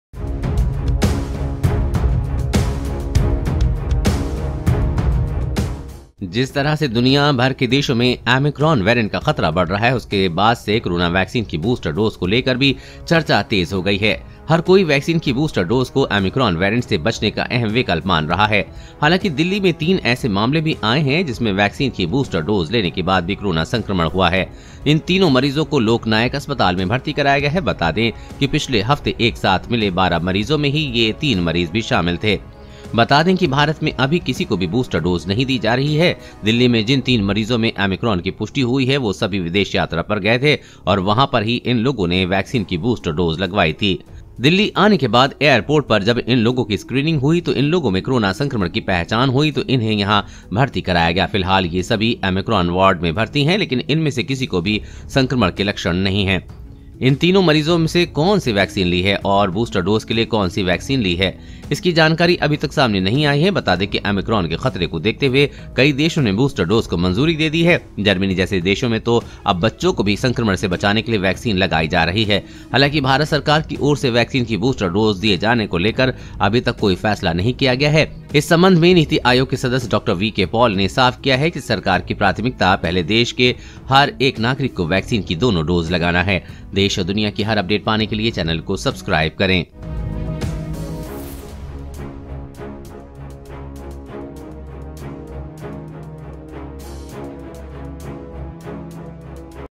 बुरी खबर ! बूस्टर डोज लेने के बाद भी भारत में 3 लोग ओमीक्रोन से संक्रमित !
सावधान ! बूस्टर डोज लेने के बाद भी भारत में 3 लोग Omicron से मिले संक्रमित

Even After Taking Booster Dose, 3 People In India Got Infected With Omicron | Corona Virus And Corona Virus Vaccine All Latest News And Updates

EDITOR ►Shadik